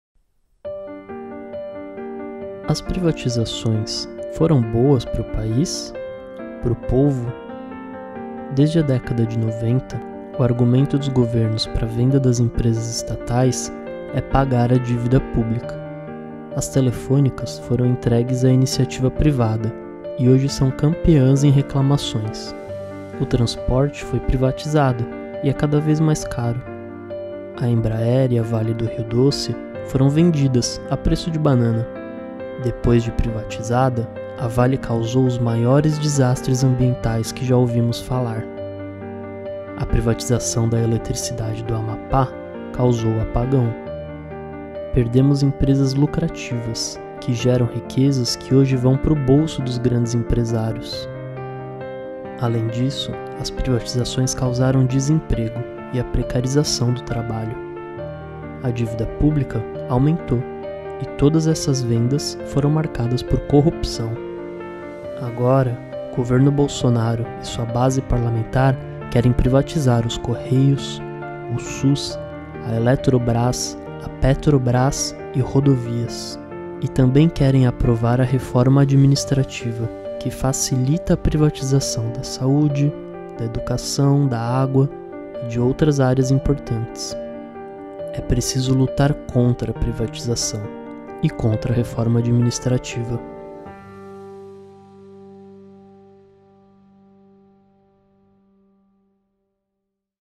As privatizações foram boas para o país?
Divulgamos aqui o excelente e fundamental vídeo produzido pelo Fórum das Entidades Nacionais dos Servidores Públicos Federais. O que você considera sobre as privatizações? Foram boas? Para quem?